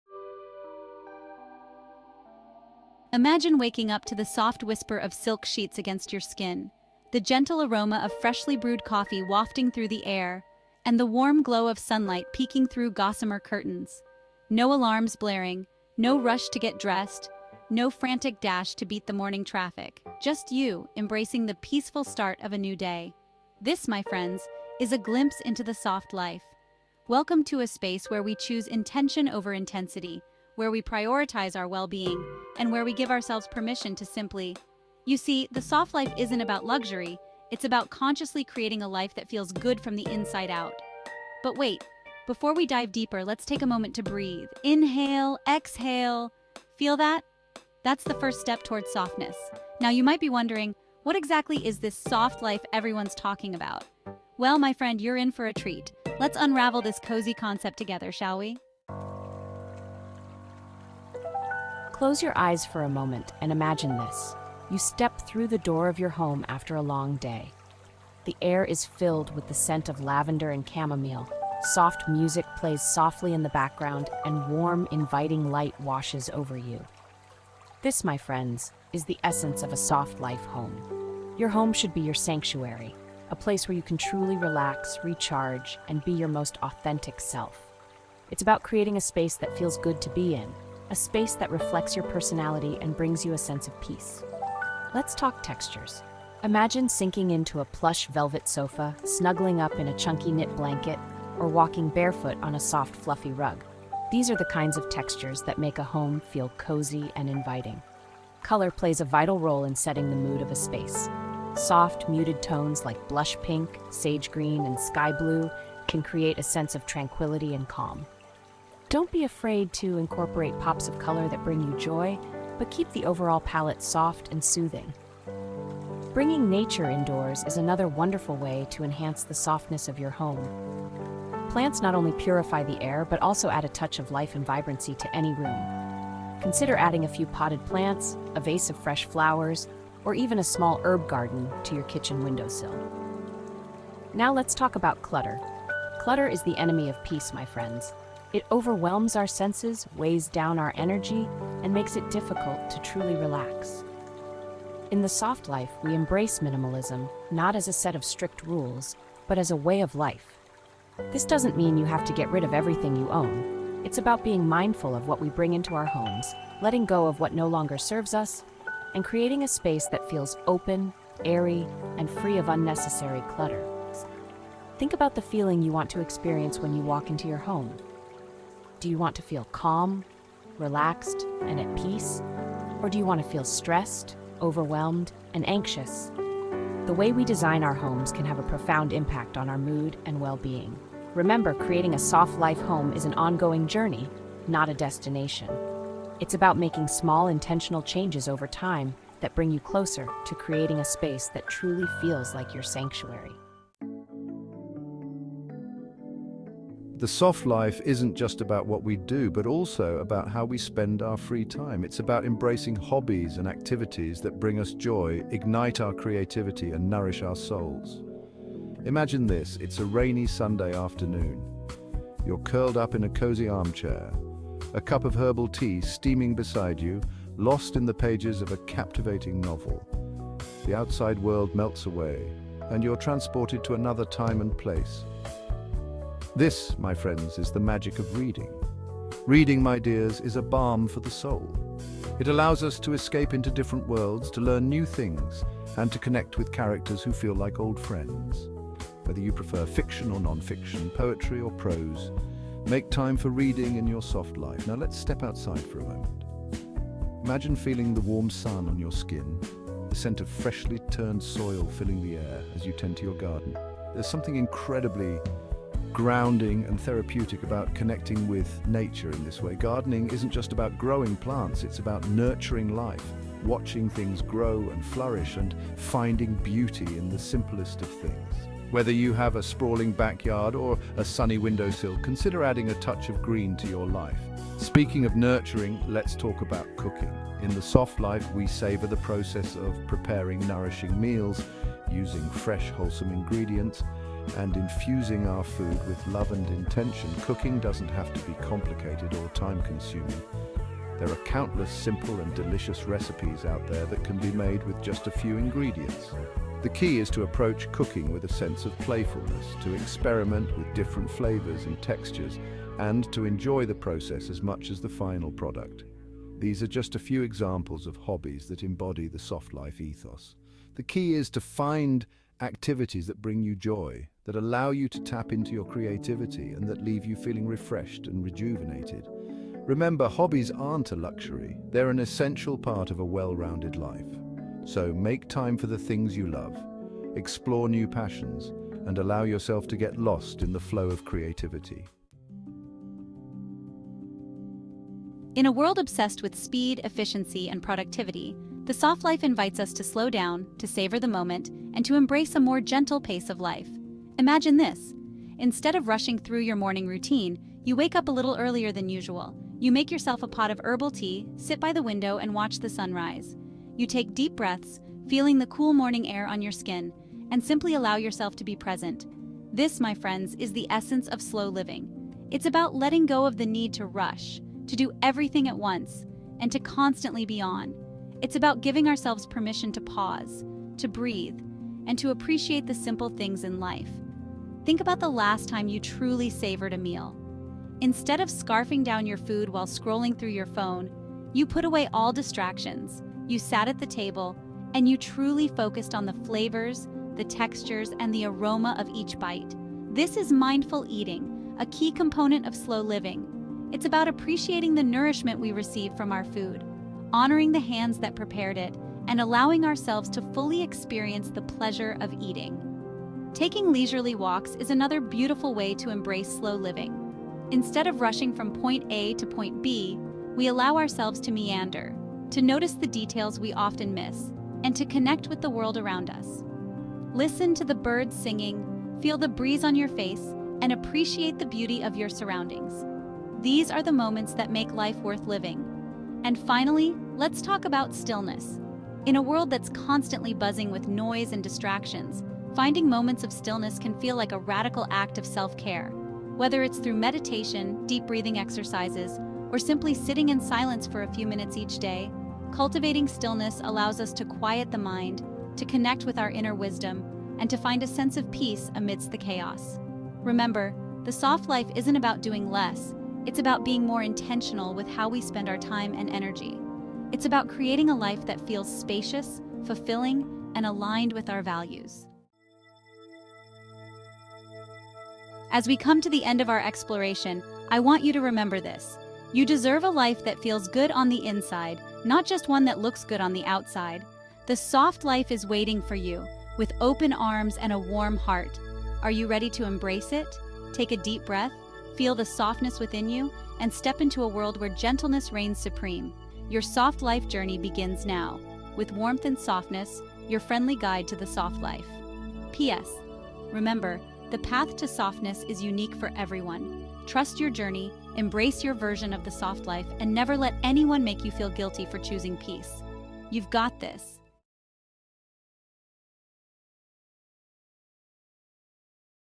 Embracing the Soft Life: A Gentle Revolution in Living
OUTLINE:

00:00:00 Welcome to the Soft Life
00:00:58 The Soft Life at Home
00:03:23 Hobbies for the Soft Life
00:05:42 Embracing the Pace of the Soft Life
00:07:52 Your Soft Life Awaits